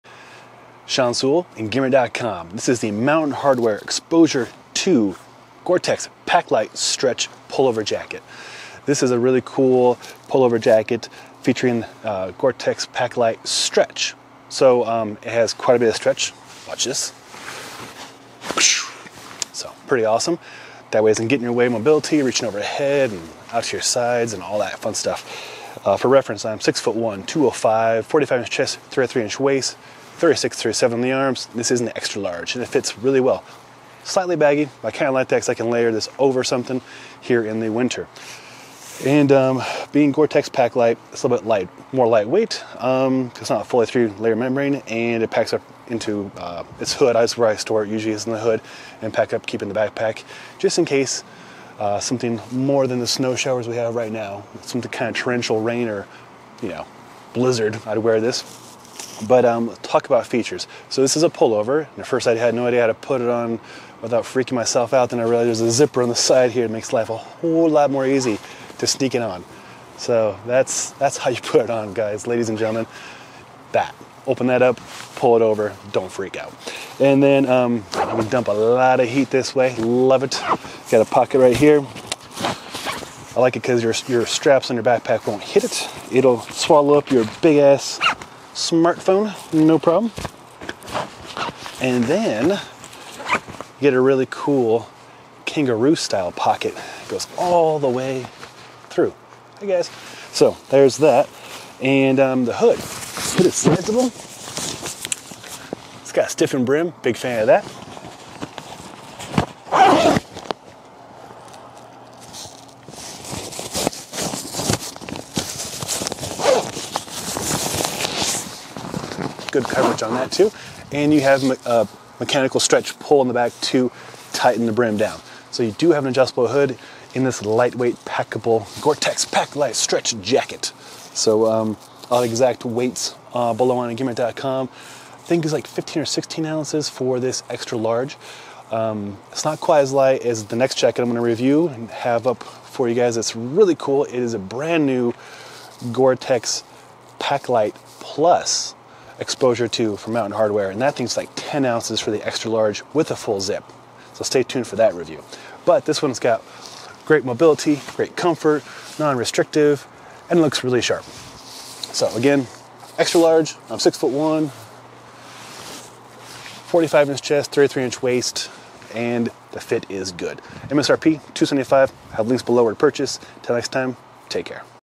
Mountain Hardwear Men's Exposure 2 Gore-Tex Paclite Stretch Pullover Review - Engearment.com
Mountain Hardwear Men's Exposure 2 Gore-Tex Paclite Stretch Pullover Review - Sean Sewell of Engearment.com

The Gore-Tex Paclite Stretch version of the Exposure pullover is pretty cool.  It fits well, has a nice hood and features a generous kangaroo pocket. I used it for some splitboarding and snow showers testing and it did great.